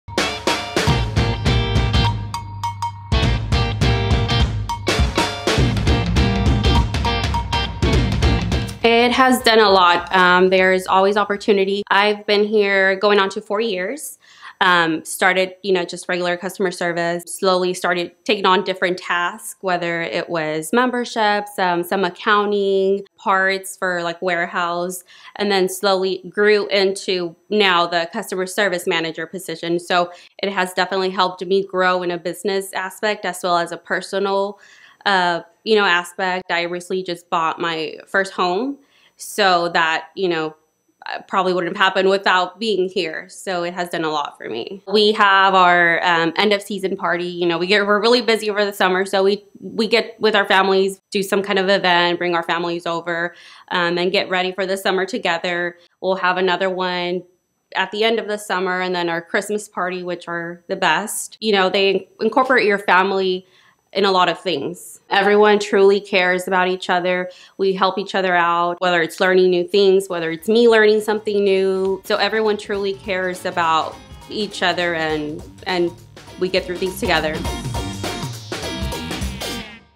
How has working at the OTE helped Valerie?
I’ve been working here going on four years and have seen a lot of growth and opportunity, both professionally and personally.